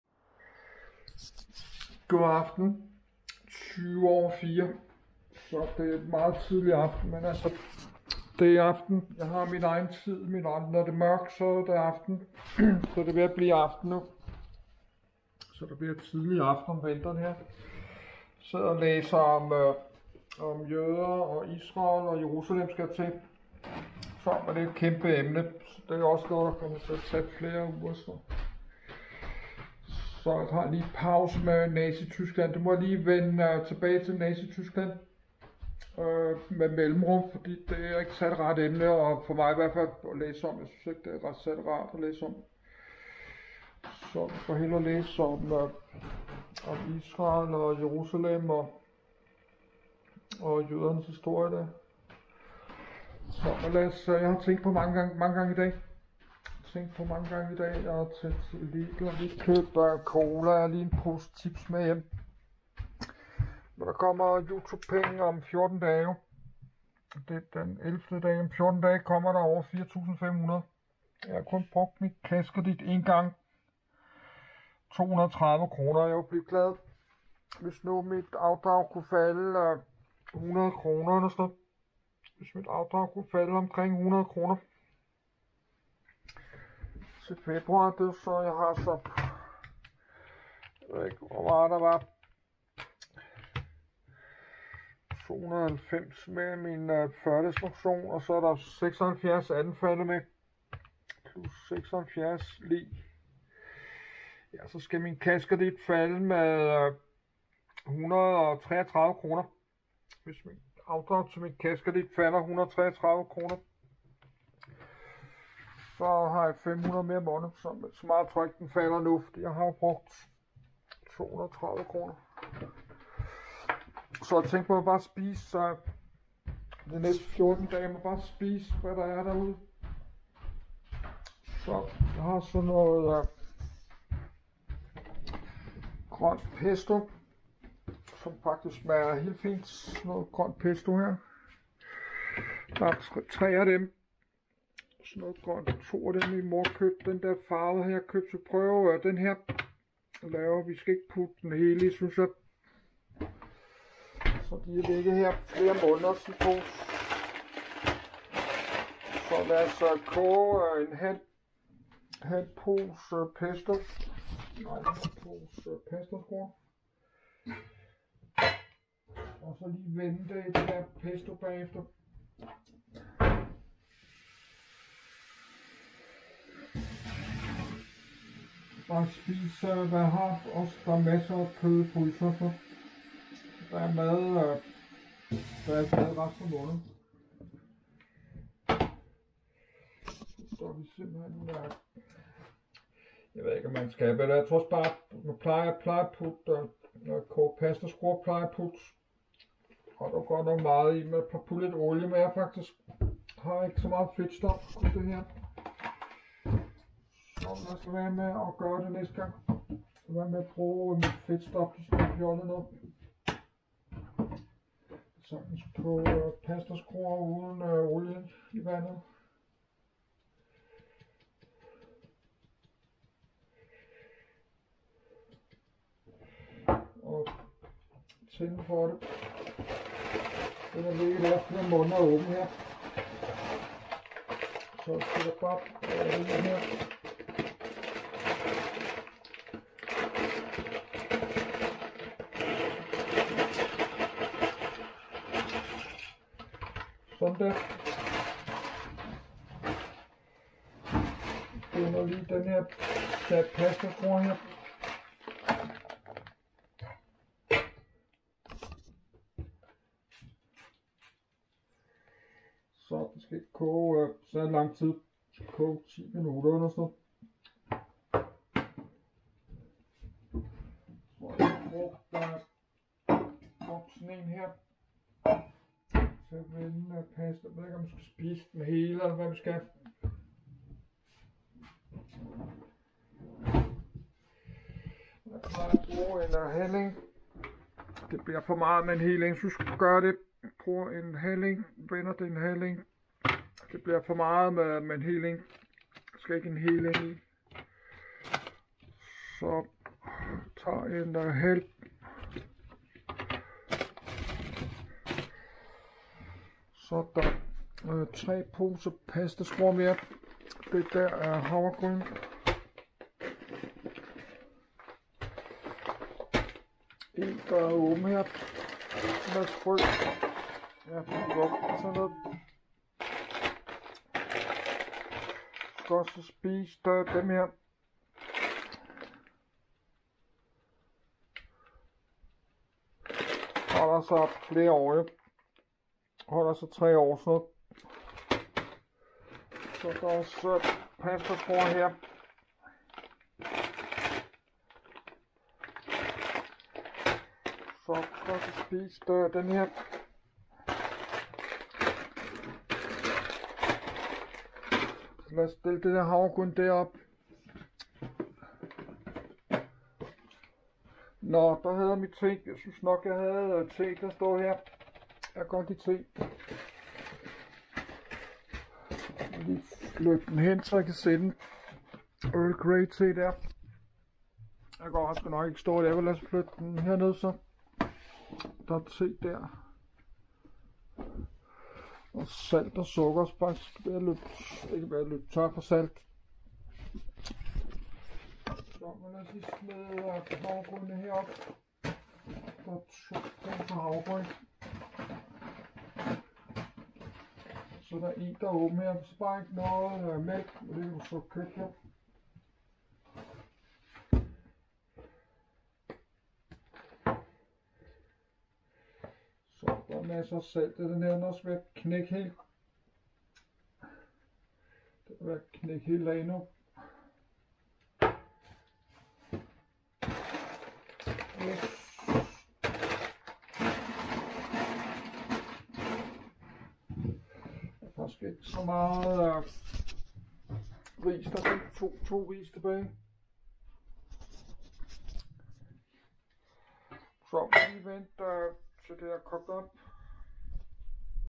MongoTV_13951 - Mongo Madskole - PASTASKRUER Med ORANGE PESTO
Velkommen til Mongo Madskole ! :-) Og god TORSDAG AFTEN. Jeg vil blive glad, hvis mit afdrag til de 2 kassekredit, kunne bare falde med omkring 100 kr. md. til den 1.Febraur :-) Men så må jeg heller ikke mere nu, bruge penge fra den kassekredit. Jeg brugt en 230 kroner af 1.000 kroner til rådighed, og der er 14-16 dage tilbage, får det næste afdrag bliver beregnet. Så jeg må bare nu, spise hvad der er i kølekabe og fryser, g i køkkenskabe.Klokken den er 17.15, og jeg er træt igen nu, og vil bare i seng, og ligge og hygge mig.